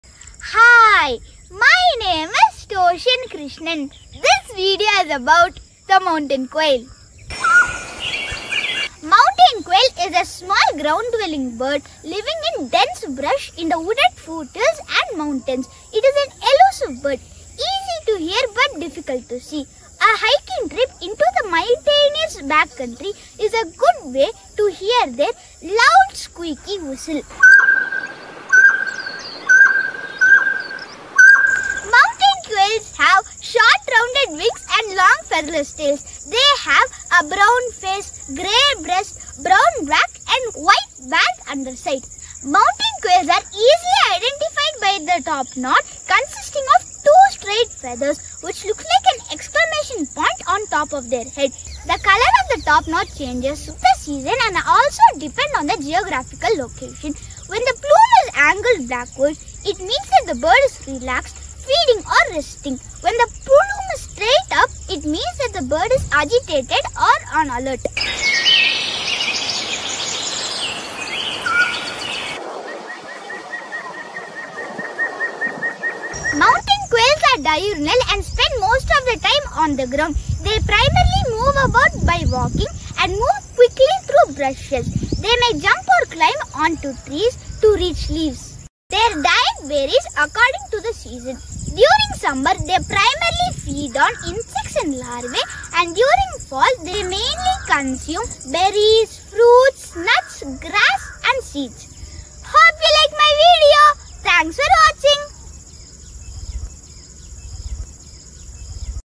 Mountain Quail
This is an informative video describing the classification, habitat and food habits of mountain quail.